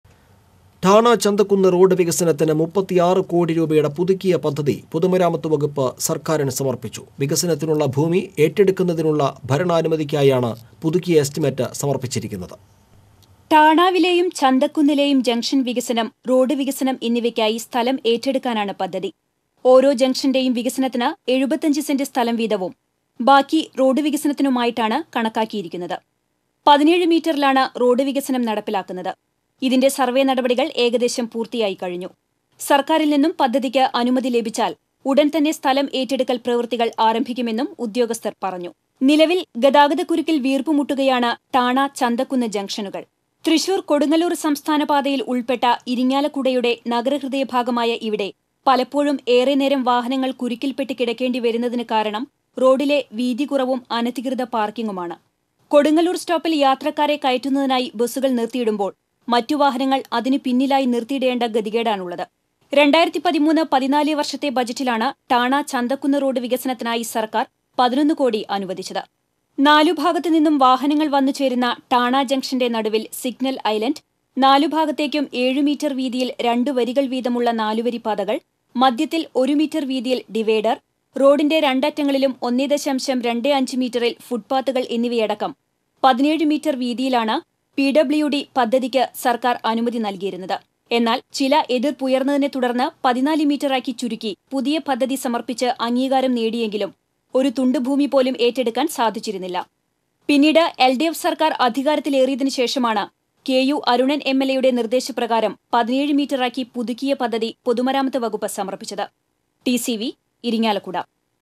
ഠാണ - ചന്തക്കുന്ന് റോഡ് വികസനത്തിന്, 36 കോടി | TCV Irinjalakuda
ഠാണ - ചന്തക്കുന്ന് റോഡ് വികസനത്തിന്, 36 കോടി രൂപയുടെ പുതുക്കിയ പദ്ധതി, പൊതുമരാമത്ത് വകുപ്പ് സര്ക്കാരിന് സമര്പ്പിച്ചു. വികസനത്തിനുള്ള ഭൂമി ഏറ്റെടുക്കുന്നതിനുള്ള ഭരണാനുമതിക്കായാണ്, പുതുക്കിയ എസ്റ്റിമേറ്റ് സമര്പ്പിച്ചിരിക്കുന്നത്.